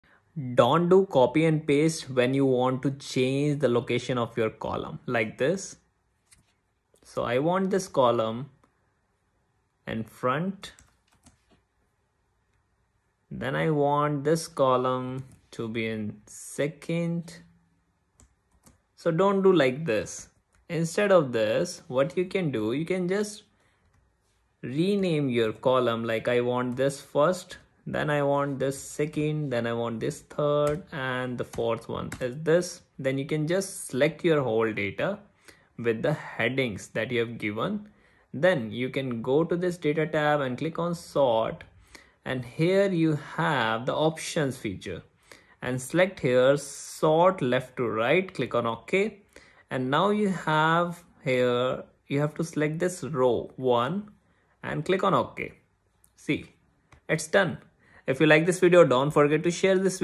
Don't do copy and paste in Excel‼️Instead Use Amazing Feature
In this video, you will learn how to sort data from left to right in Excel.